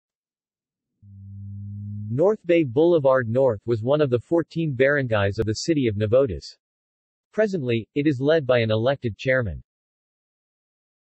Northbay Boulevard North | Wikipedia audio article
This is an audio version of the Wikipedia Article:
Northbay Boulevard North

SUMMARY
Northbay Boulevard North  was one of the fourteen barangays of the City of Navotas. Presently, it is led by an elected Chairman.